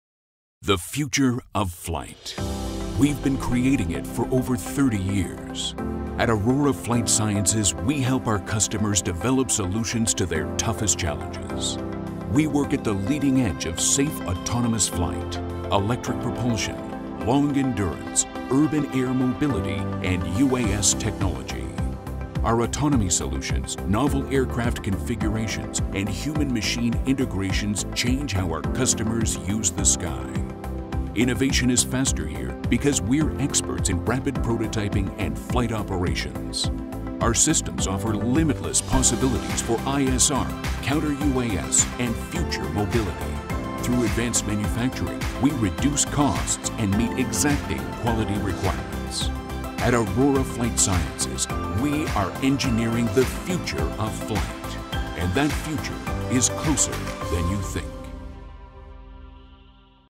Aurora Flight Sciences: Engineering the Future of Flight
Aurora Flight Sciences, a Boeing Company, advances the future of flight by developing and applying innovations across aircraft configurations, autonomous systems, propulsion technologies, and manufacturing processes. With a passionate and agile team, Aurora delivers solutions to its customers’ toughest challenges while meeting high standards of safety and quality.